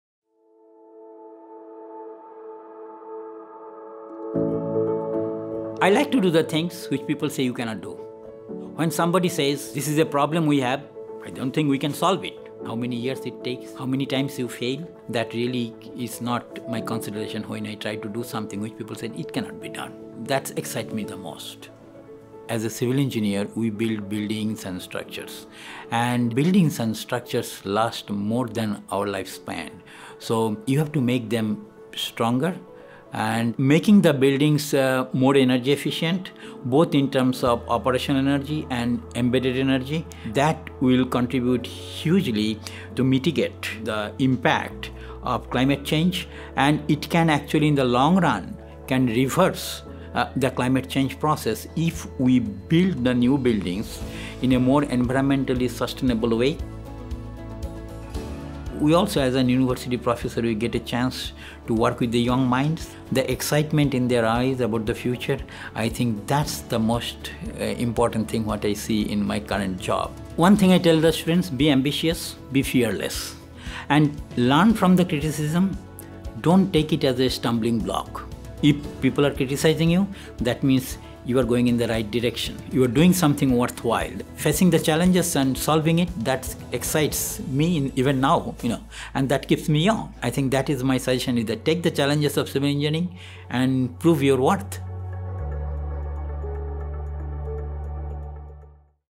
2023 The Meritorious Achievement Award - Phalguni Mukhopadhyaya, PhD, P.Eng.
The recipient of the Meritorious Achievement Award, Dr. Phalguni Mukhopadhyaya, P.Eng., sets research into action. A professor in the Civil Engineering Department at the University of Victoria, Phalguni's highly regarded research has sparked building envelope advancements in moisture management and thermal insulation around the world.